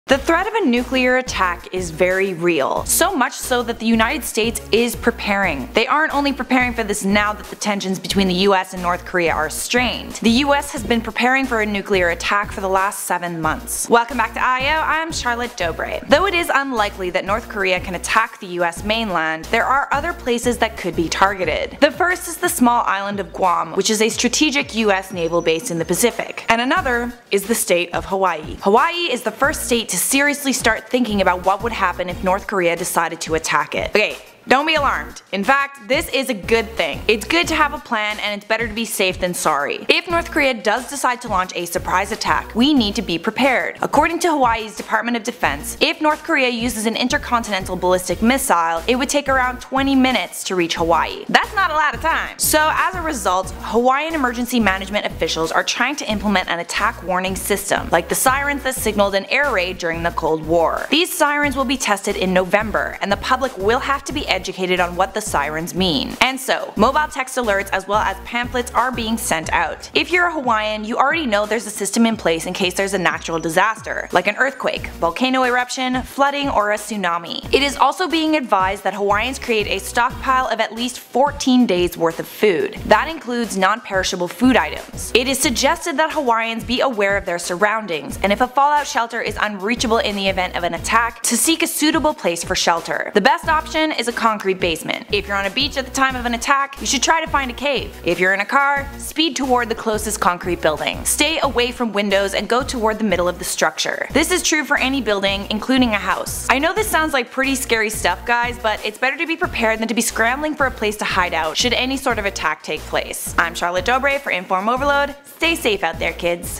The United States Prepares For A Nuclear Attack
Hawaii prepares as North Korea threat grows More than 70 years later, the threat of a nuclear strike is being assessed and planned for just in case, although US officials dialed back rhetoric on Wednesday.

2408 Lakeshore Blvd W.